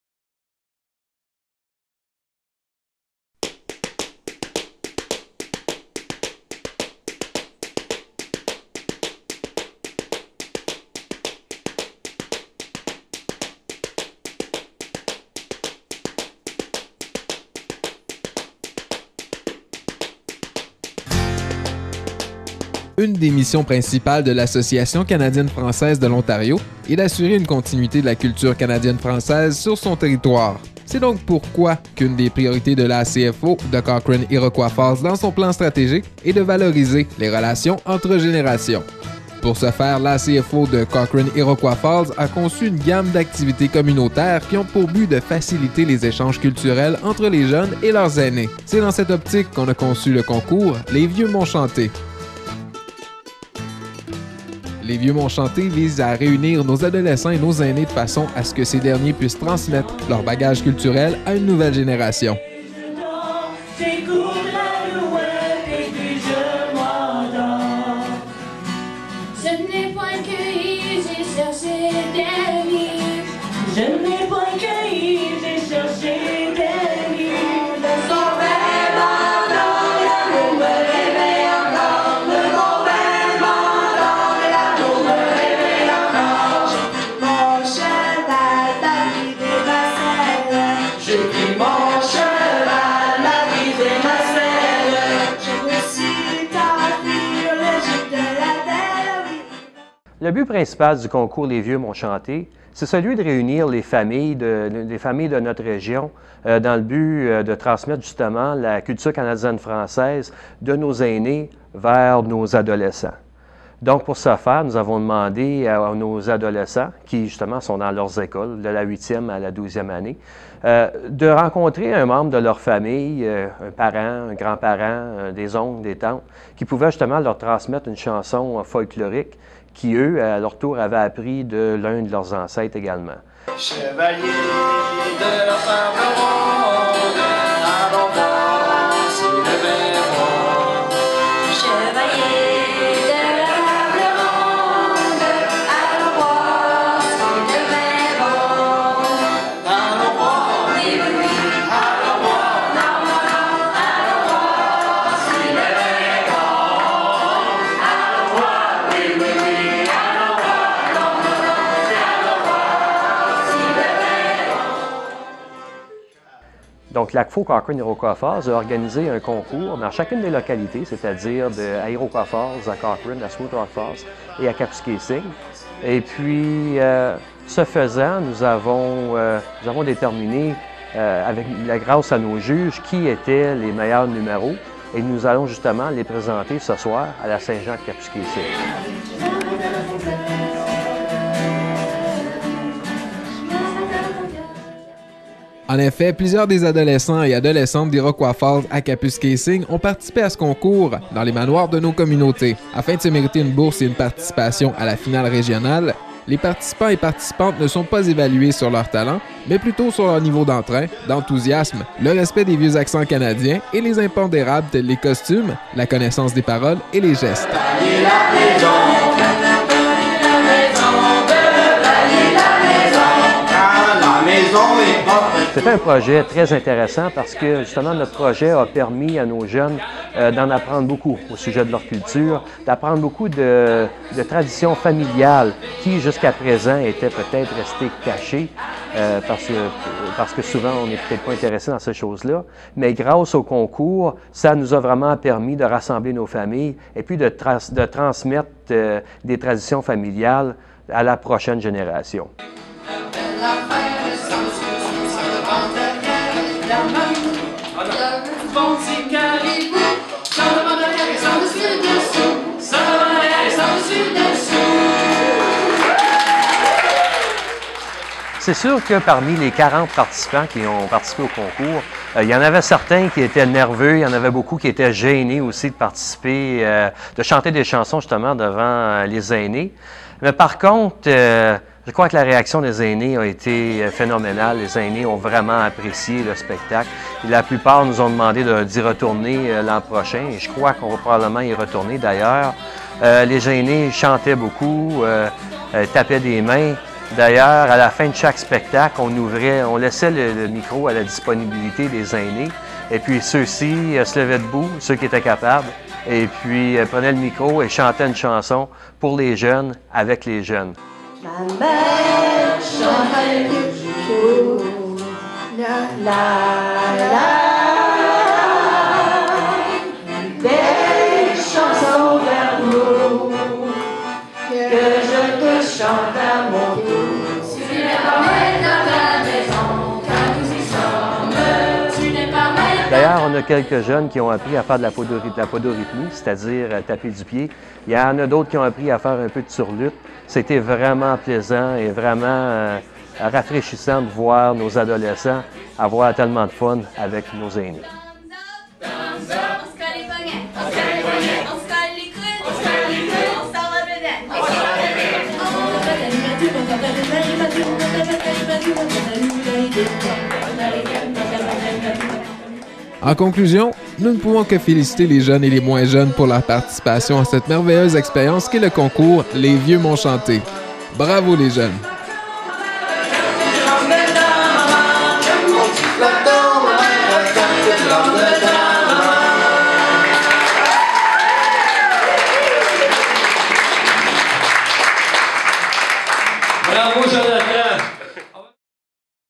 Les Vieux m'ont chanté
Description du projet "Les Vieux m'ont chanté".  Une initiative de l'ACFO Cochrane -- Iroquois Falls qui vise à réunir nos adolescents et nos aînés de façon à ce que ces derniers puissent transmettre leur bagages culturel à une nouvelle génération.